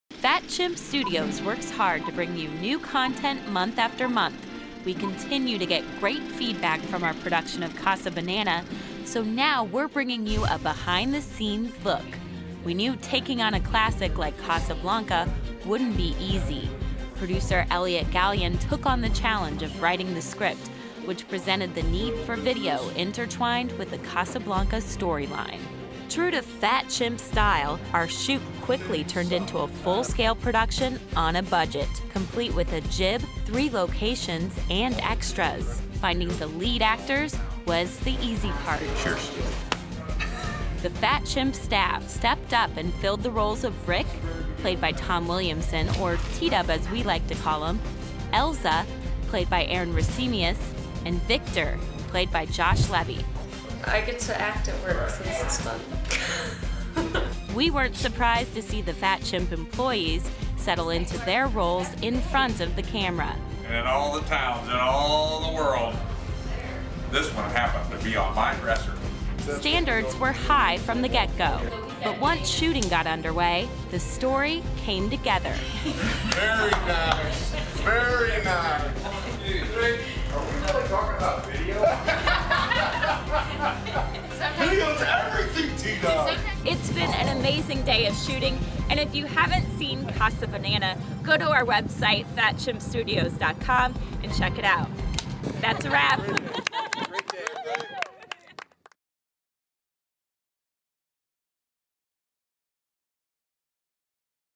Casabanana: Behind the Scenes
Go behind the scenes of Fat Chimp Studio's popular Casabanana video. Find out how we brought Victor, Ilsa and Rick to life.

Brooke Russell: Fat Chimp Studios works hard to bring you new content month after month. We continue to get great feedback from our production of Casabanana, so now we're bringing you a behind the scenes look. We knew taking on a classic like Casablanca wouldn't be easy. Producer, Elliot Gallion, took on the challenge of writing the script, which presented the need for video intertwined with the Casablanca storyline. True to Fat Chimp style, our shoot quickly turned into a full scale production on a budget complete with a jib, three locations and extras. Finding the lead actors was the easy part.

T-Dubb: Cheers.

Brooke Russell: The Fat Chimp staff stepped up and filled the rolls of; Rick, played by Tom Williamson, or T-Dubb as we like to call him, Ilsa played by Erin Resimius and Victor, played Josh Levey.

Erin: I get to act at work, so this is fun.

Brooke Russell: We weren't surprised to see the Fat Chimp employees settle into their rolls in front of the camera.

T-Dubb: In all the towns, in all the World, this one happened to be on my dresser.

Brooke Russell: Standards were high from the get-go. But once shooting got underway, the story came under way.

Kevin: Very nice, very nice. Video is everything T-Dubb.

Brooke Russell: It has been an amazing day of shooting and if you haven't seen Casabanana, go to our website, fatchimpstudios.com, and check it out.  That's a wrap.